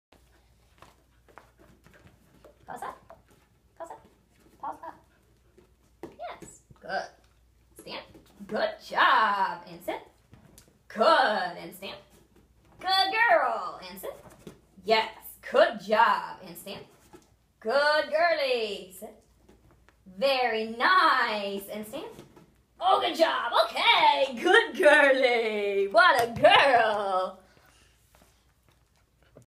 Pogo squats on K9FITbone and play ball
An advanced squat because of the amount of stabilization required to keep the ball steady.